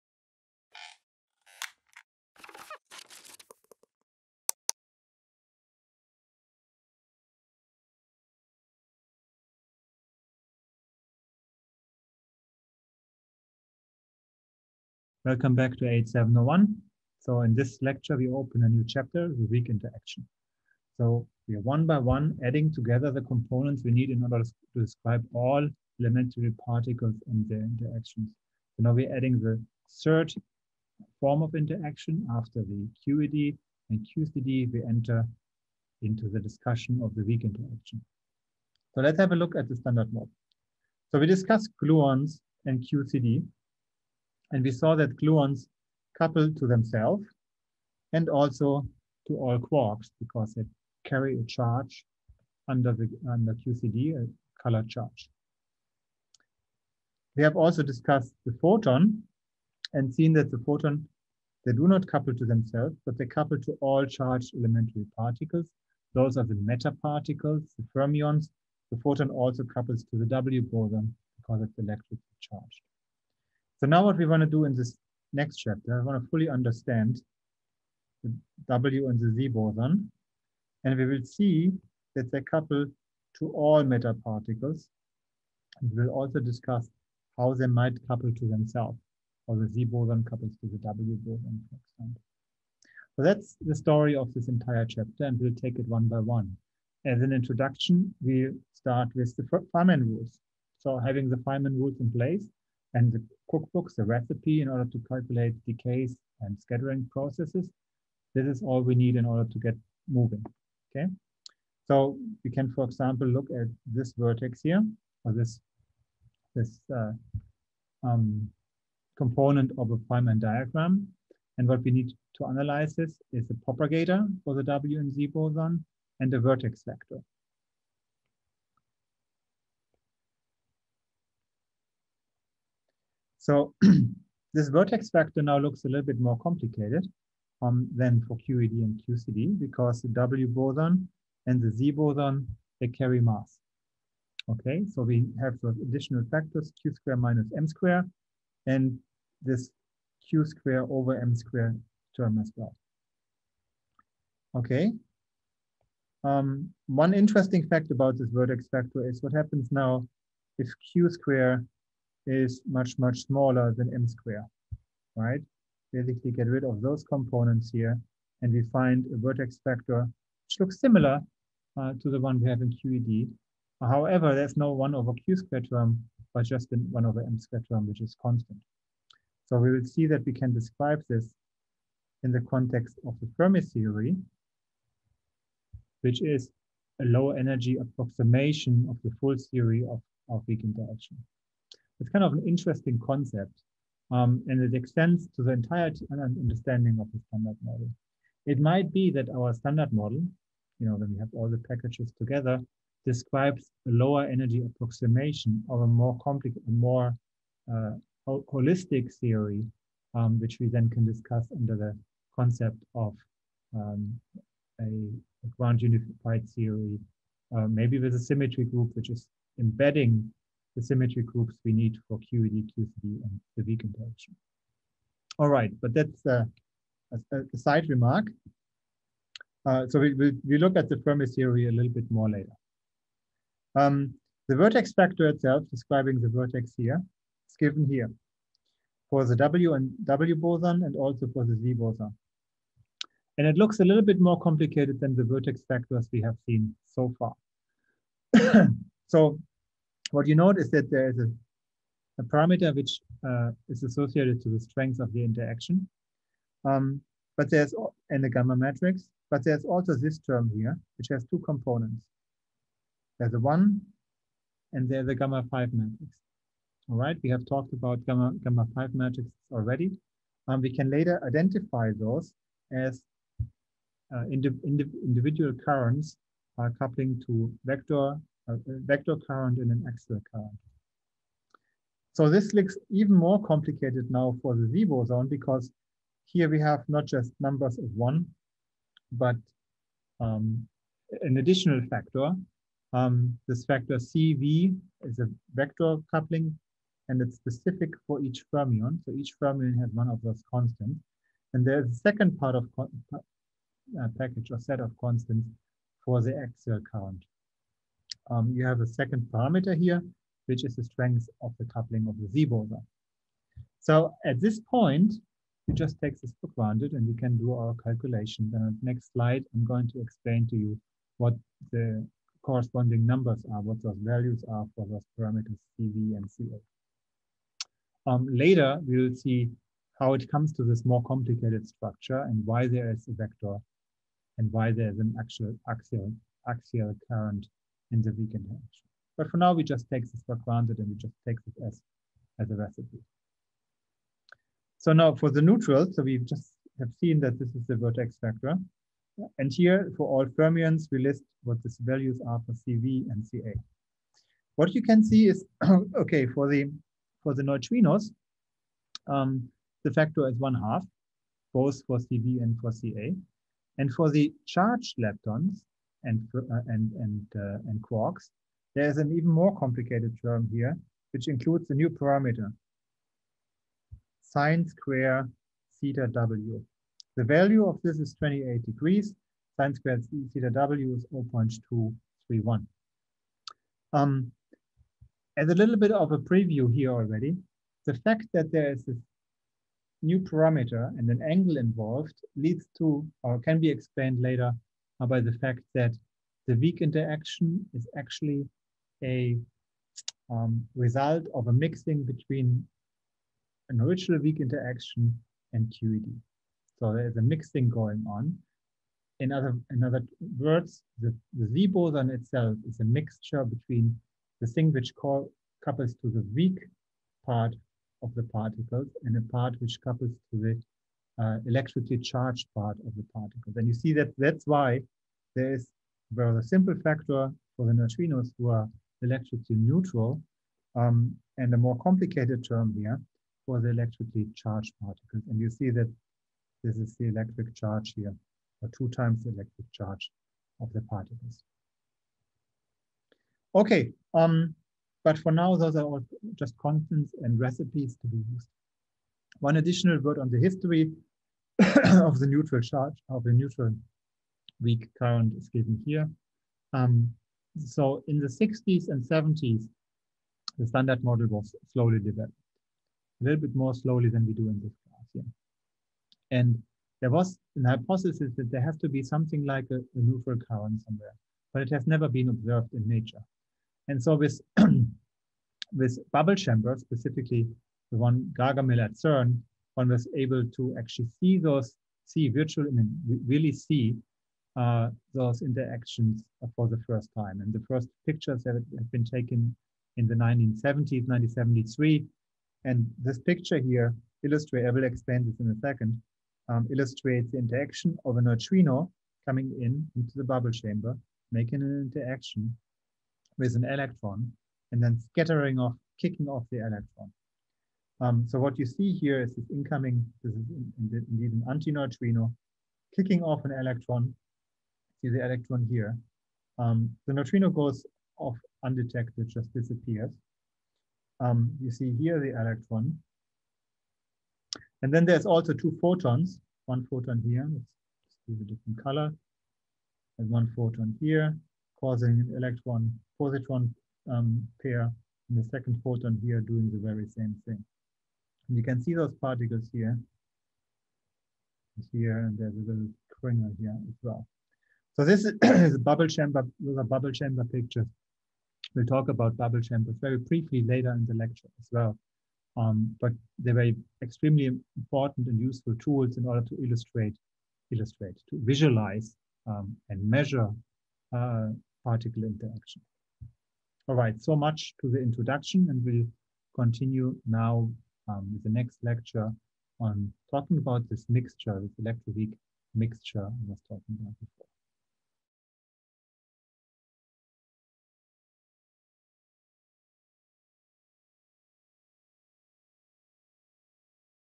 L6.1 Weak Interactions: Feynman Rules
MIT 8.701 Introduction to Nuclear and Particle Physics, Fall 2020

Introduction to the Feynman Rules of the weak interaction.